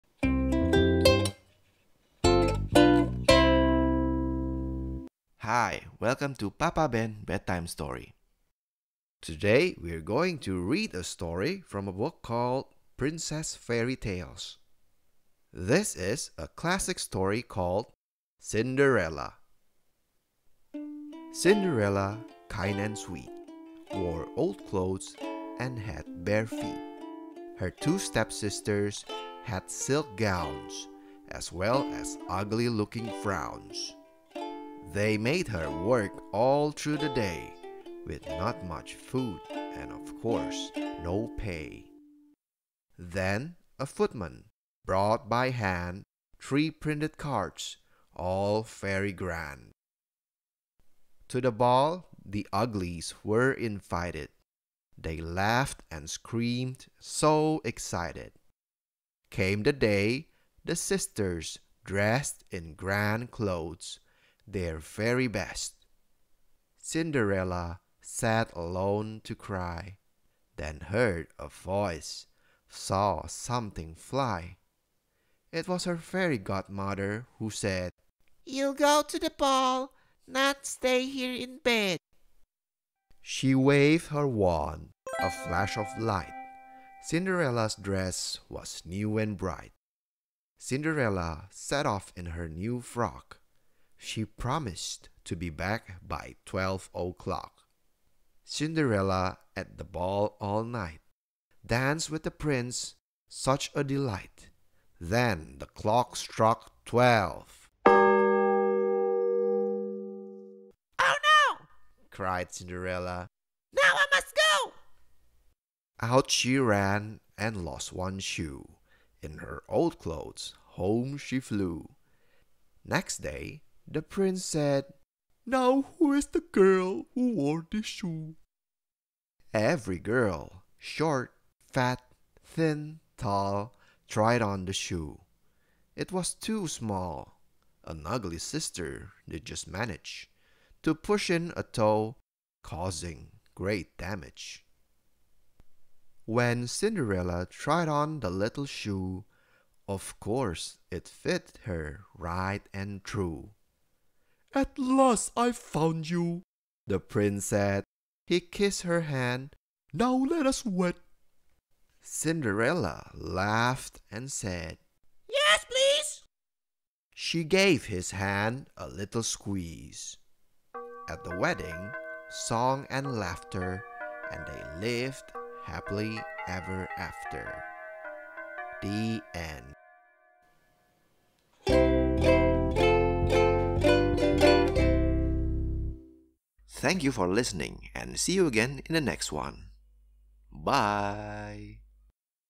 Cinderella | Children Bedtime Story | Bedtime Story for Kids
Cinderella is a classic folk story that has been retold & rewritten so many times. We can trace the root of the story to the ancient Greece.

This story version is taken from a children story book called Princess Fairy Tales  (ISBN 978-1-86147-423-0) published by ARMADILLO Anness Publishing (annesspublishing.com) in 2015.

All rights to the story & illustration are belong to the publisher/writer/illustrator.

I hope you enjoy our rendition of the story.